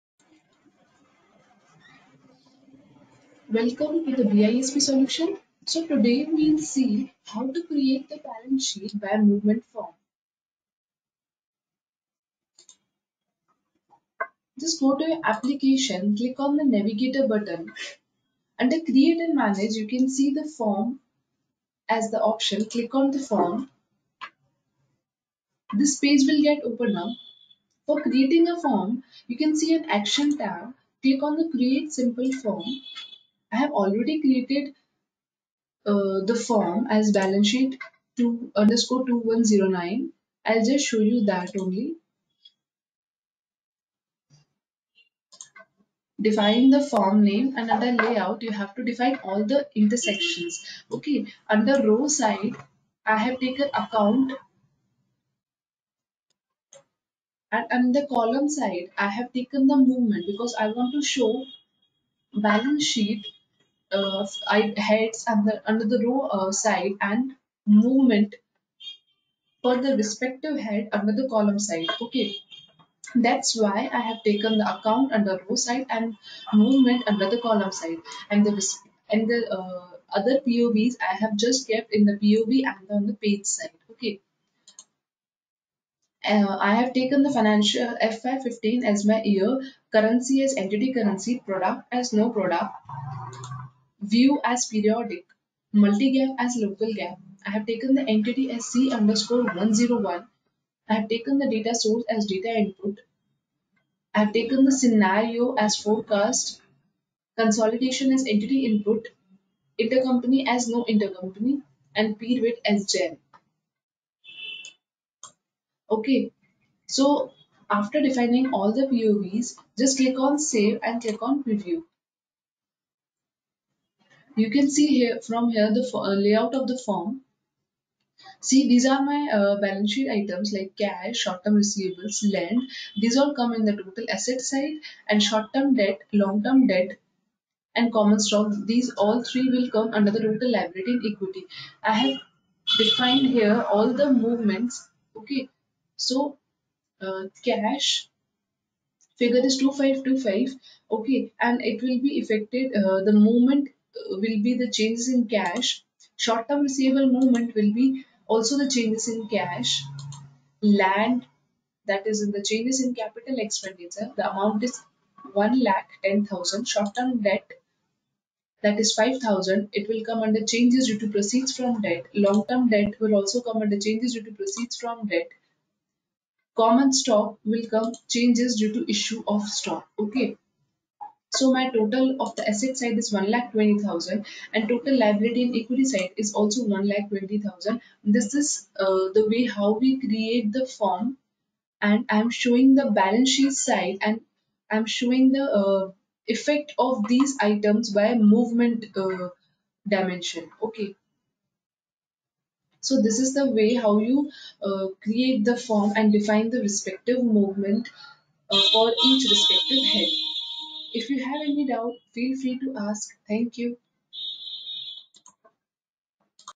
Oracle FCCs Balance Sheet by Movement Data form | Oracle Financial Consolidation | FCCS training
This video is all about  Oracle FCCs Balance Sheet by Movement Data form.